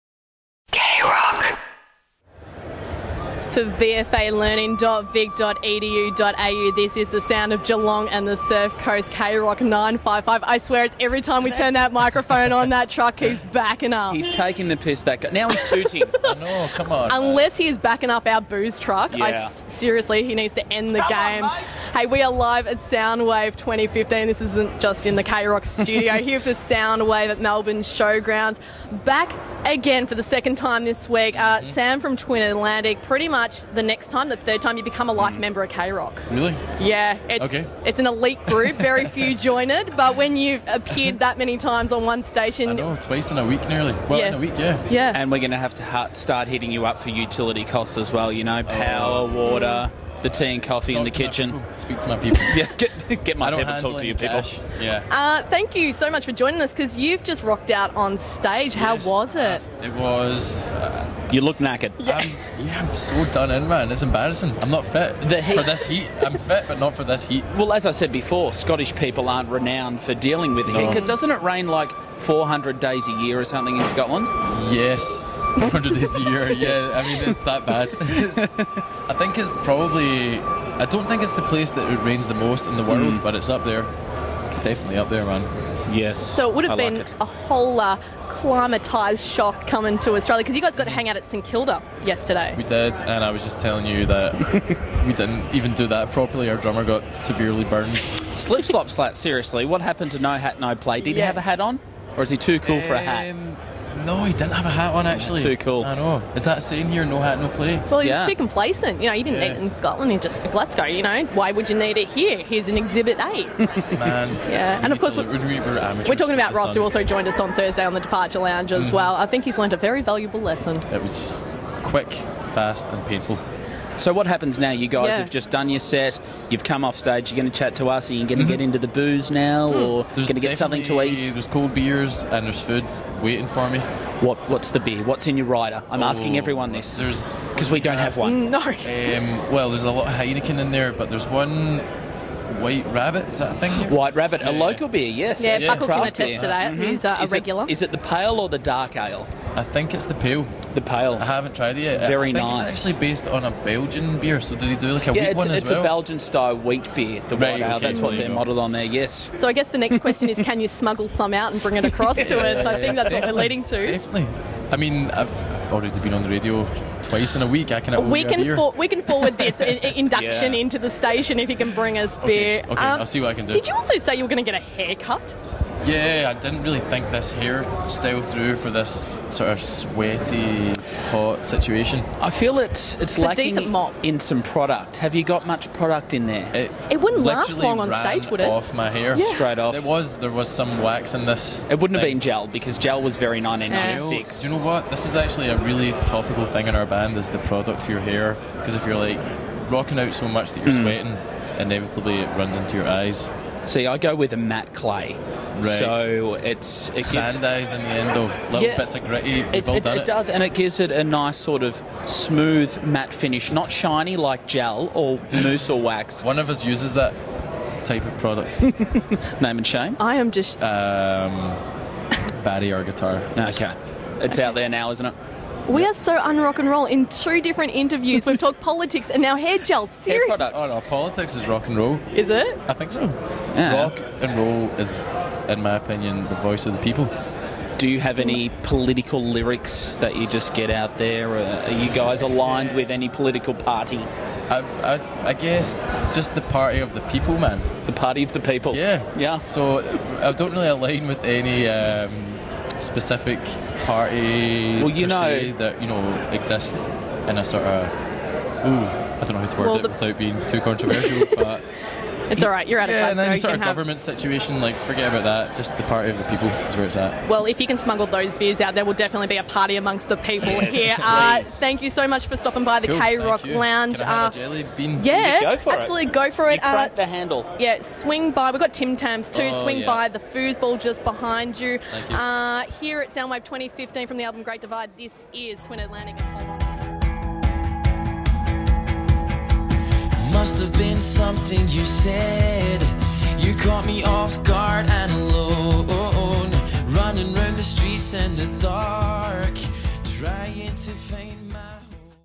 K-Rock Interviews Sam from Twin Atlantic LIVE at Soundwave 2015
K-ROCK 95.5 broadcasting live from backstage Soundwave Melbourne 2015. Interviewing Sam from Twin Atlantic.